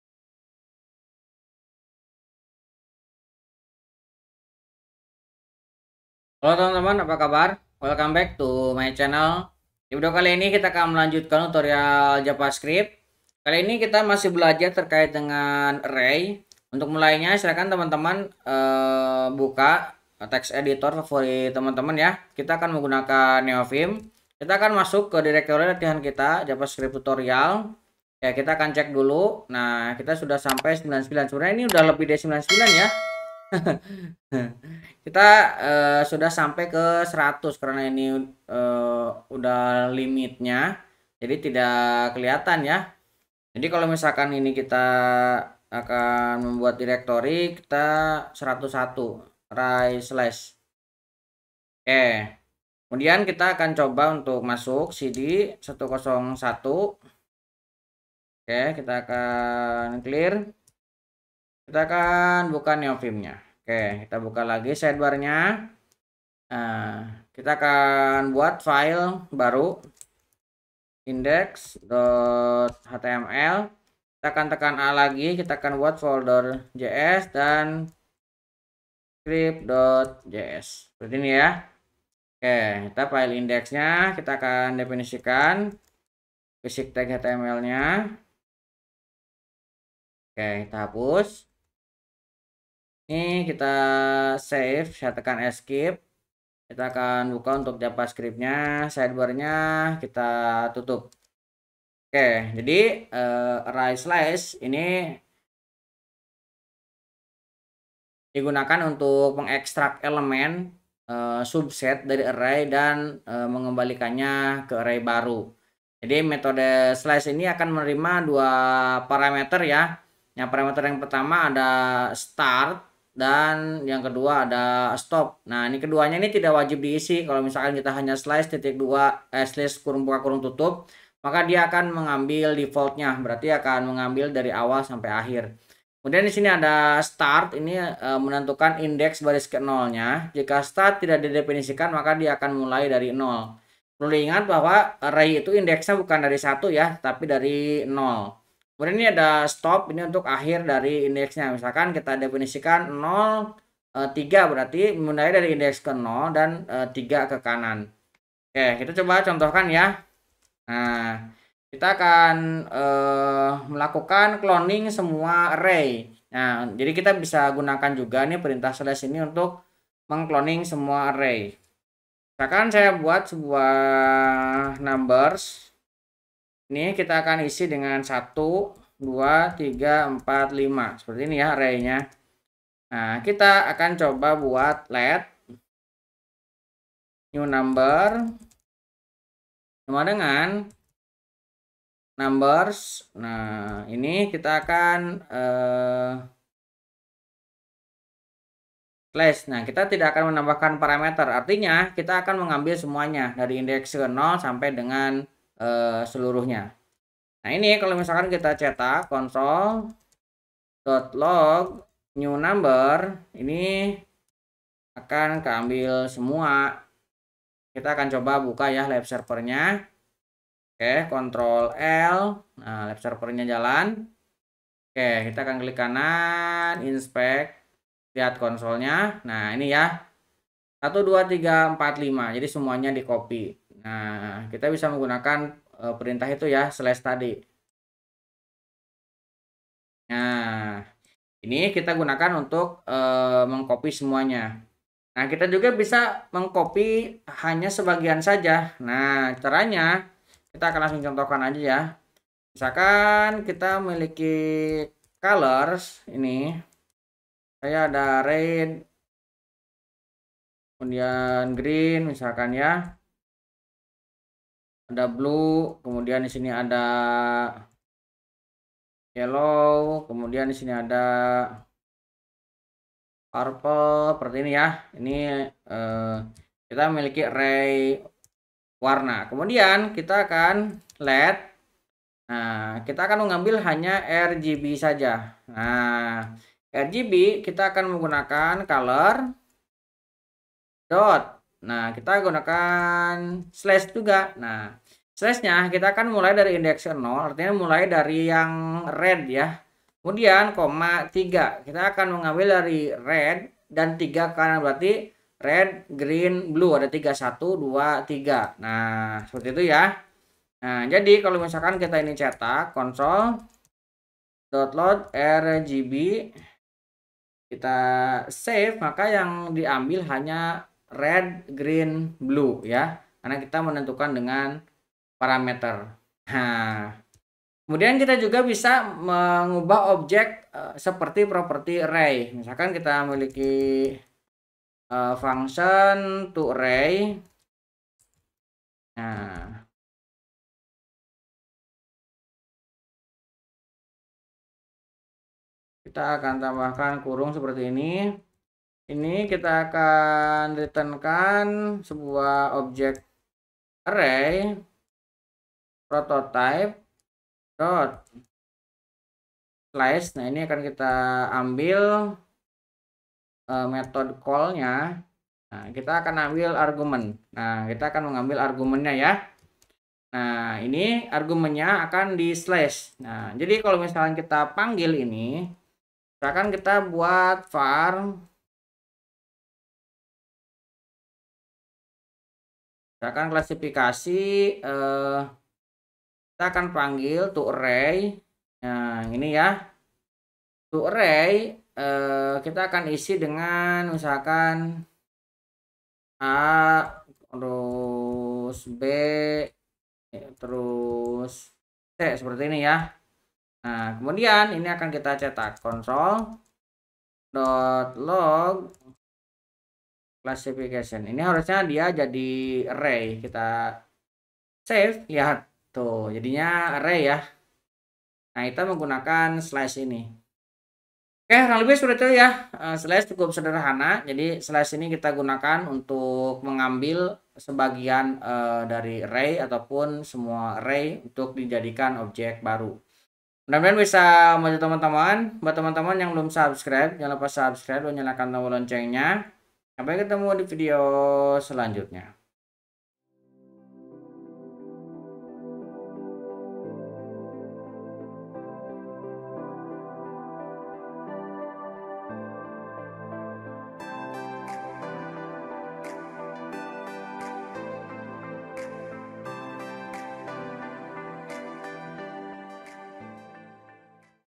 JAVASCRIPT TUTORIAL #101 ARRAY SLICE
Hallo teman-teman video kali ini kita akan memulai tutorial untuk JAVASCRIPT, materi ini benar-benar dari dasar, jadi cocok untuk kamu yang pemula.

Terima Kasih